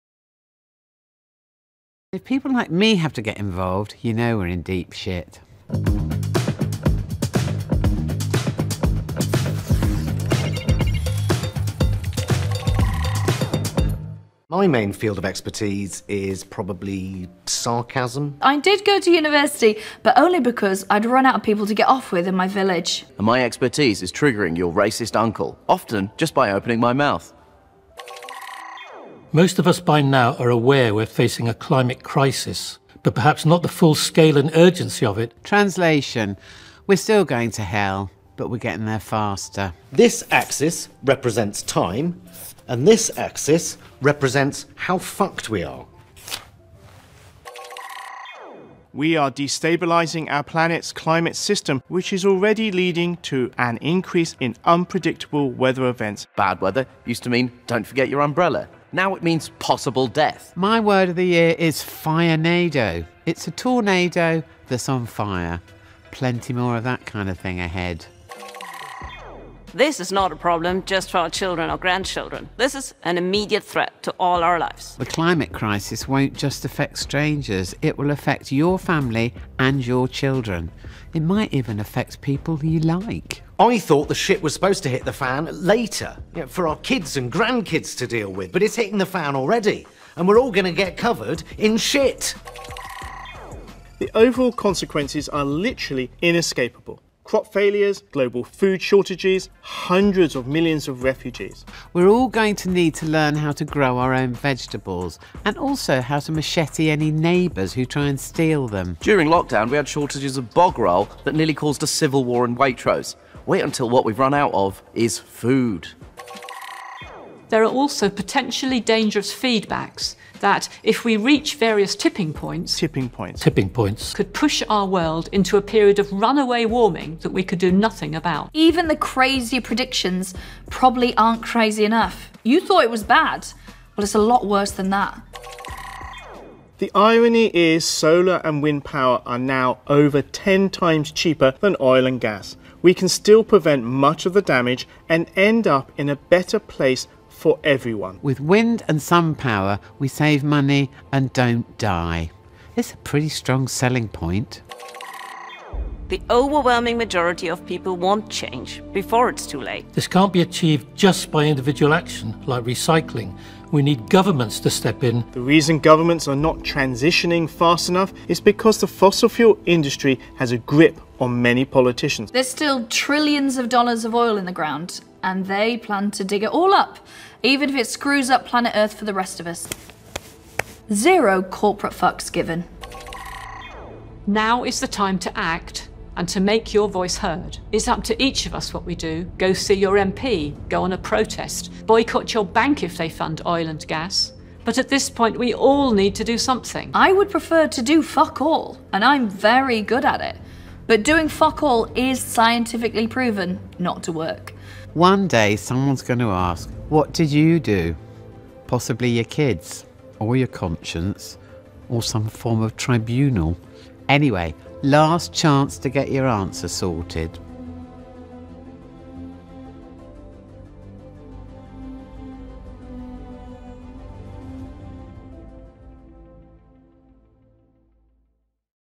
Climate Science Translated; All-Stars Version
Watch star comedians help top climate scientists translate science into human, and cut through to the masses!
After 10 million views, we've made an All-stars version.